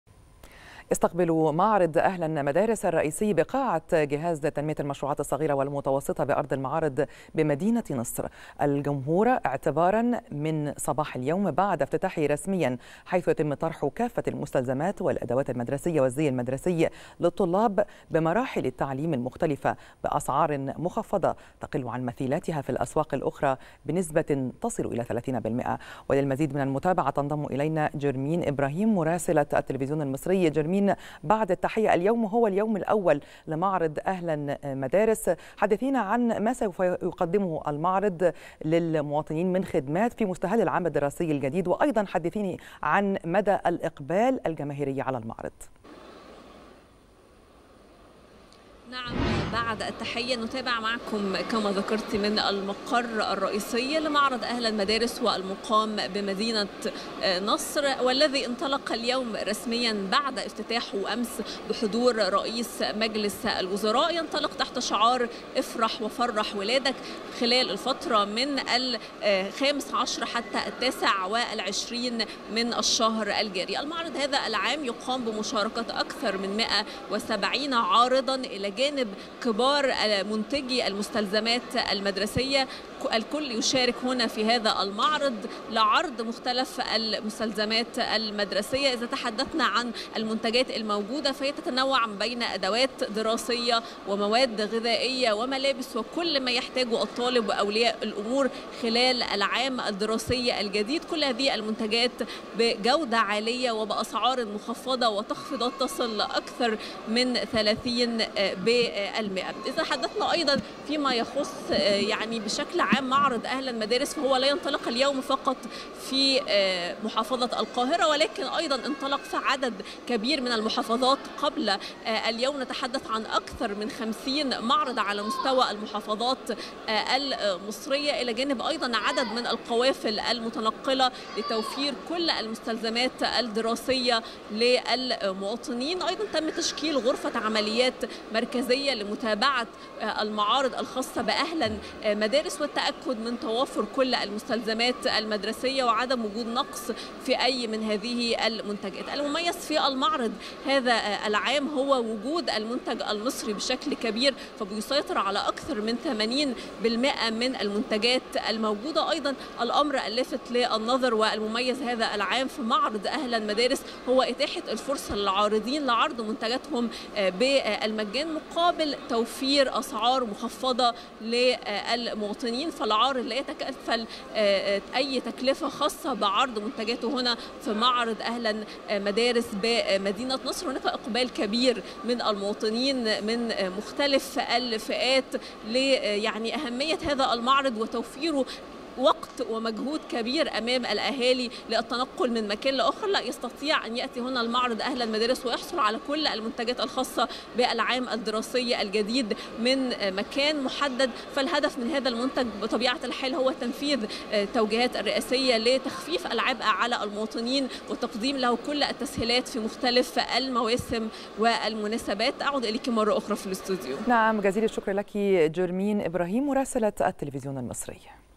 طرح كافة المستلزمات والأدوات المدرسية بتخفيضات كبيرة ضمن مبادرة أهلا مدارس
مراسلة التليفزيون المصري ترصد تفاصيل طرح كافة المستلزمات والأدوات المدرسية بتخفيضات كبيرة ضمن مبادرة أهلا مدارس